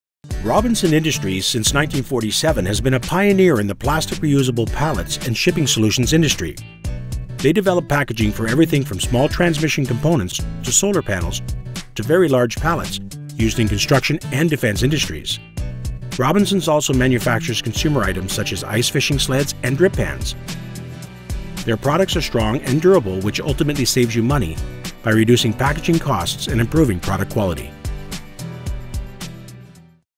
Robinson Industries Inc
Robinson Industries manufacture plastic injection molded, structural foam, single and twin sheet thermoformed pallets, containers and dunnage as well as some consumer items.  We prototype, tool, sheet extrude, recycle, supply urethanes, plastic corrugated and standard and custom totes and knockdown containers.